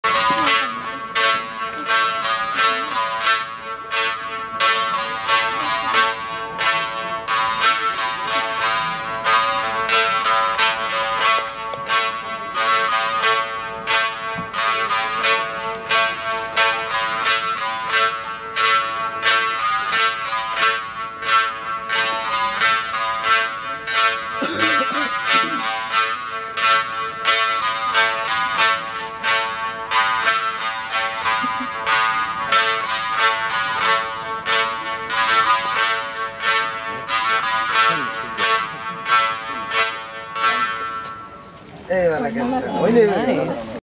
Young Kyrgyz Talent!
Girls playing traditional Kyrgyz music at the Tenth Anniversary of The Christensen Fund in Central Asia Reception